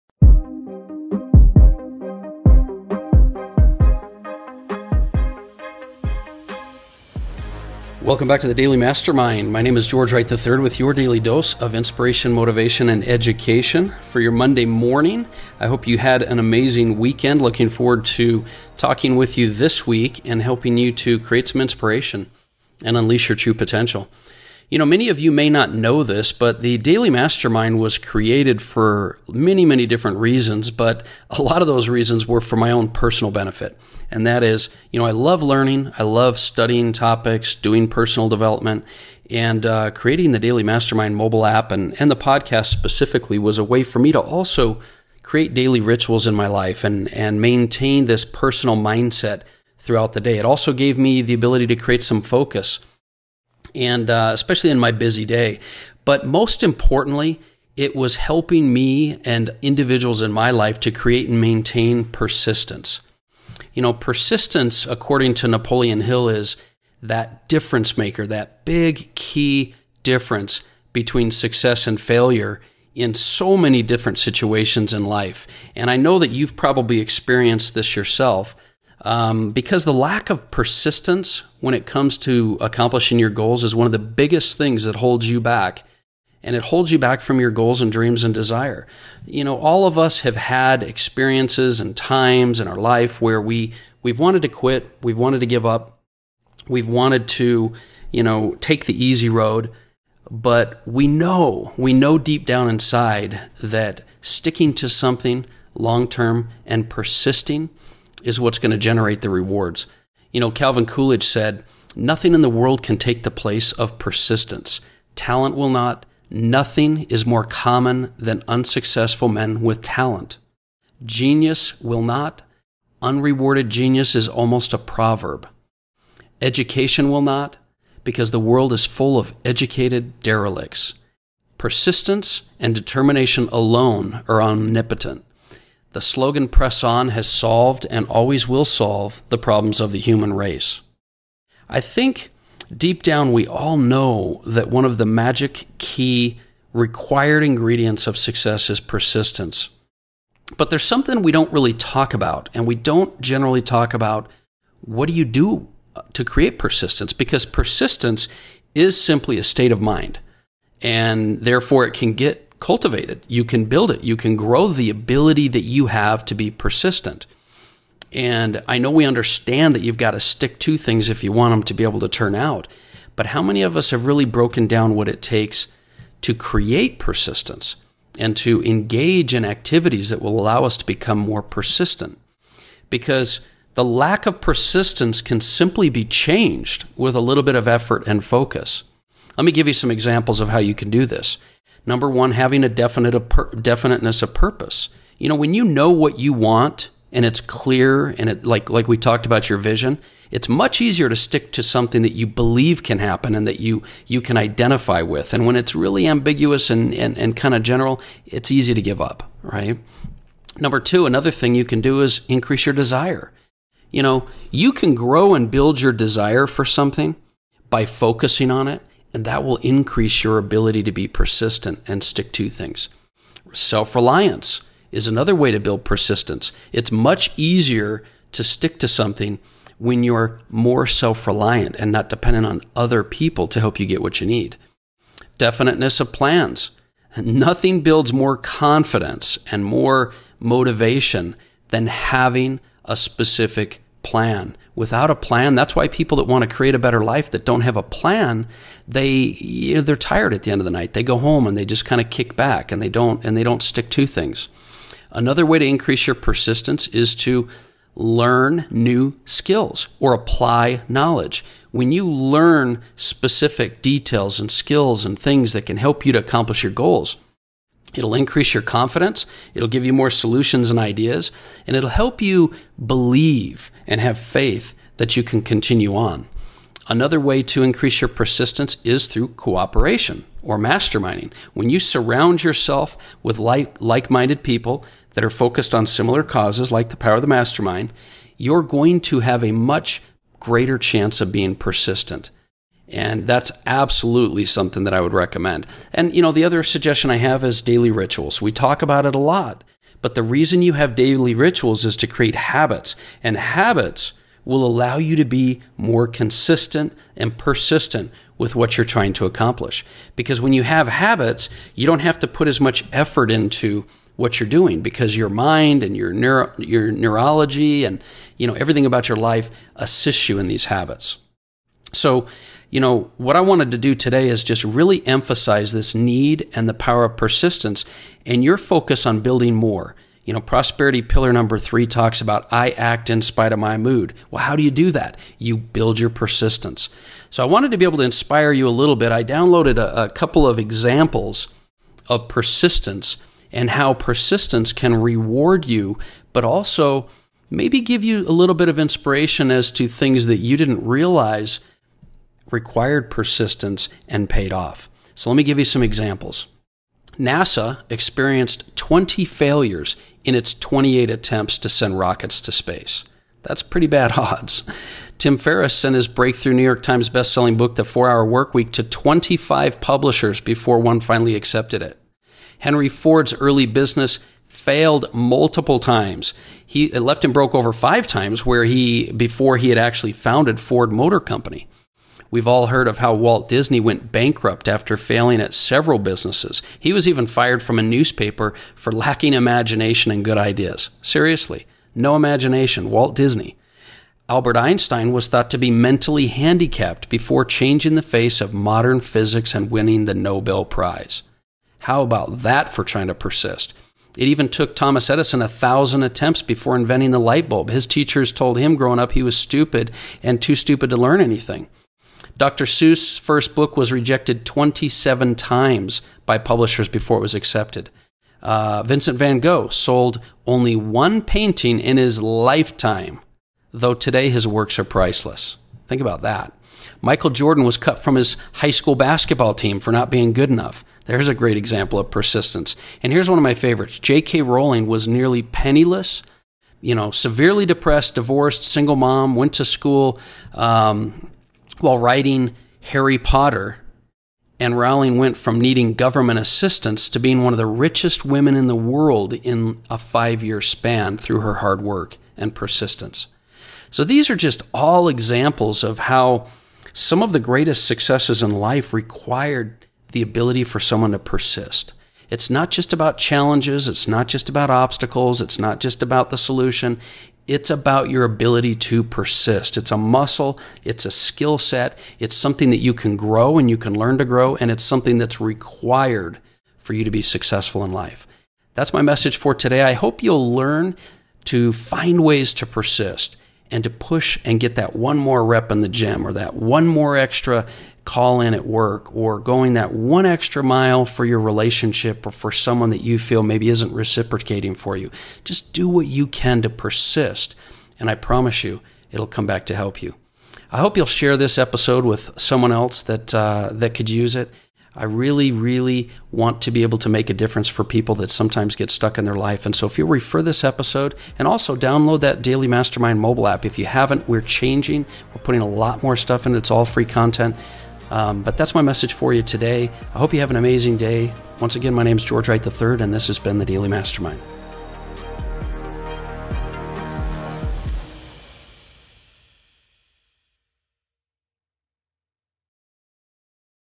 Tips to Learn How to be More Persistent
You may not know this, but I started the Daily Mastermind for myself in order to help me learn, grow and stay more consistent in my life.

Today I want to talk to you about persistence and why it is so important to your success.  Many of us don't realize that you can grow your ability to be persistent.

I hope you enjoy this topic. Thanks for listening.

George Wright III